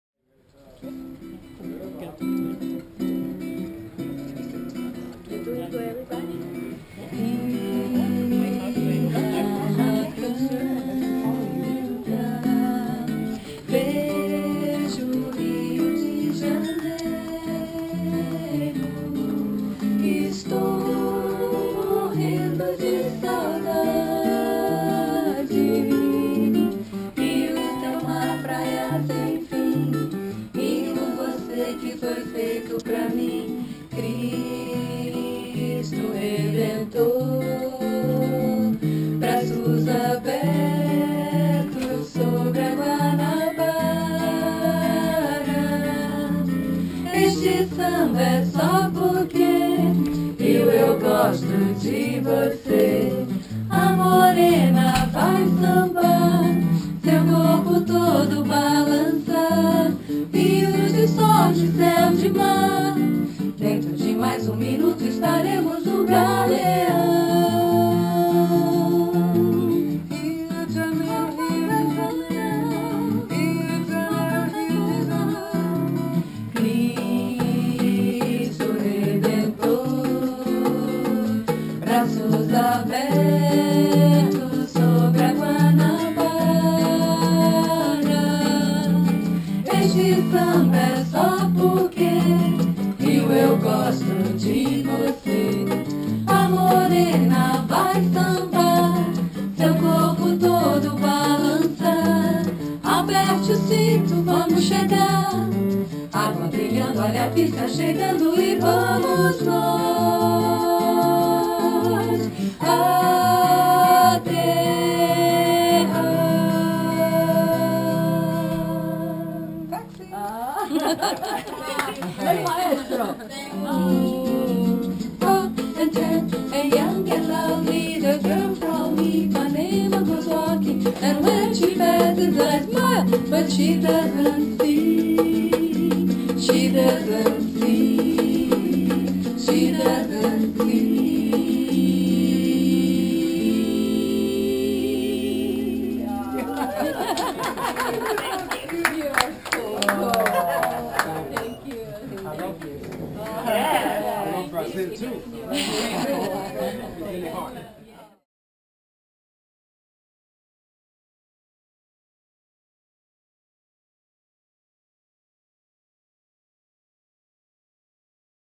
Brazilian Voices - Arts & Healing Program
More than 200 people were delighted with the melodies and beauty of Bossa Nova performed by Brazilian Voices at Cleveland Clinic and Sylvester Comprehensive Cancer Center on September 22nd.